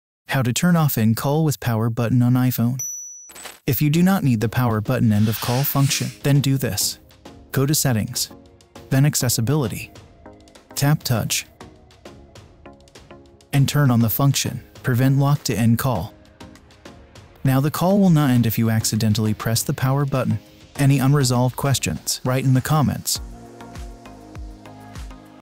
How to Turn OFF End Call with Lock Button on iPhone?
Simple and very short step-by-step video How to Turn OFF End Call with Lock Button on iPhone!

If you do not need the power button end of call function, then do this:
then accessibility
tap touch
and turn on the function
prevent lock to end call
Now the call will not end if you accidentally press the power button
00:00 Intro
00:03 Turn OFF End Call with Power Button